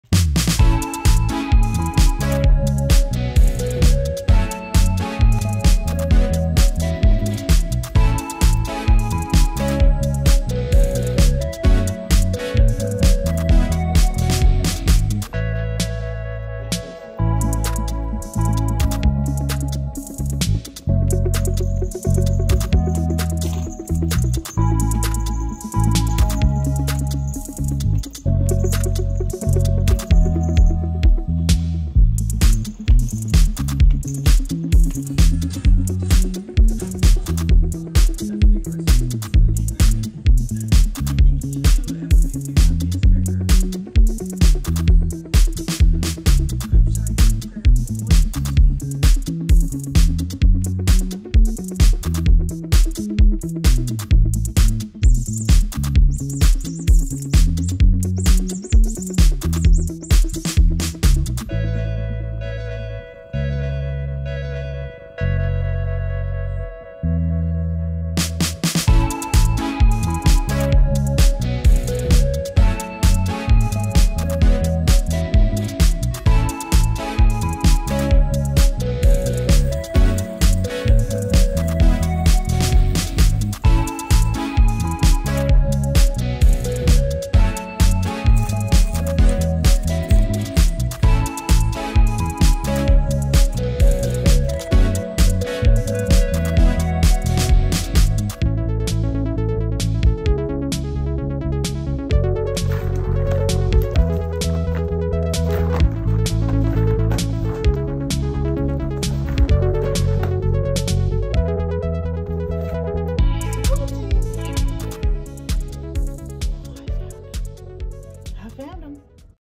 Tuxedo cats are cute and a little crazy
Meet Watson. My crazy and yet cute, tuxedo cat.

Cute and playful clips of Watson.